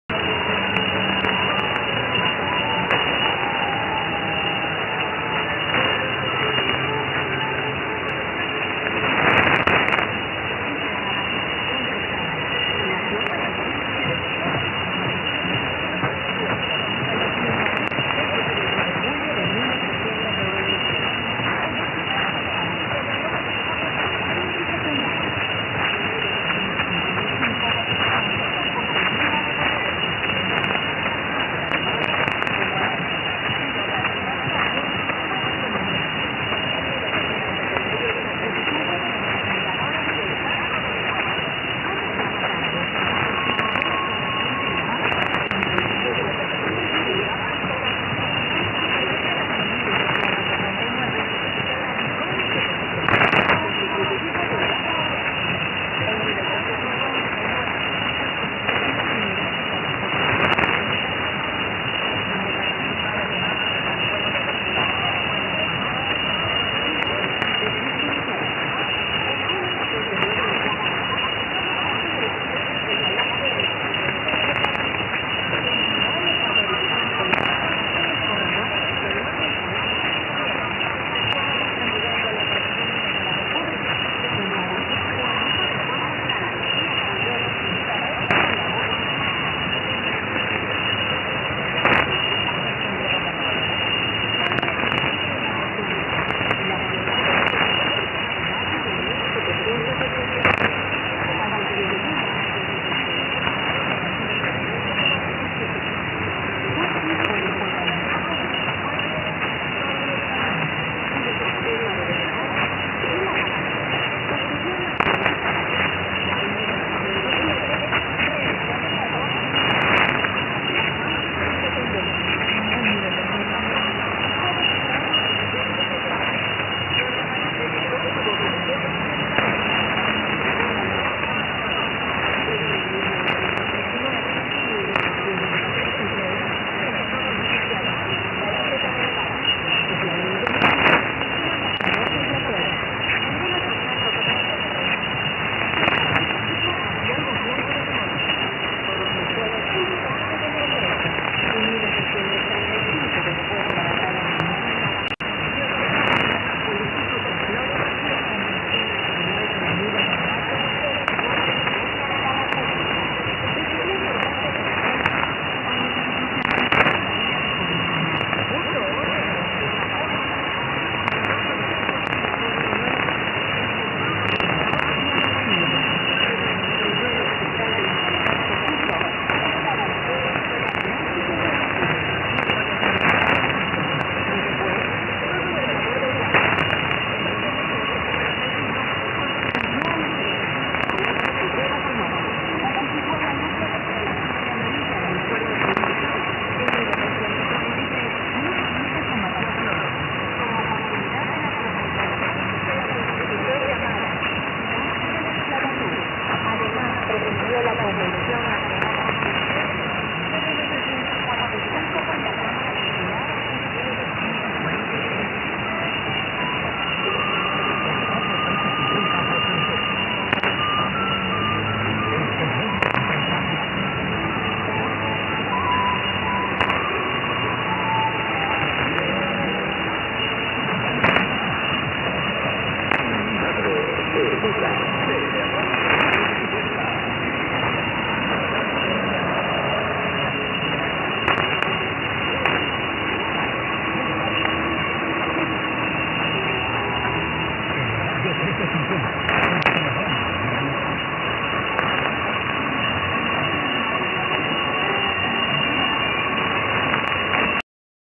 MW DX: Teleradio Guayaquil 1350 kHz received in Germany
HCVR2 Teleradio 13-50 from Guayaquil, Ecuador on 1350 kHz received in Germany on 15 September 2016 at 0431 UTC.

Receiver: Elad FDM-S2 with Wellbrook ALA1530 active loop antenna (indoors)

Distance: 10184 km  -  Power: 1 kW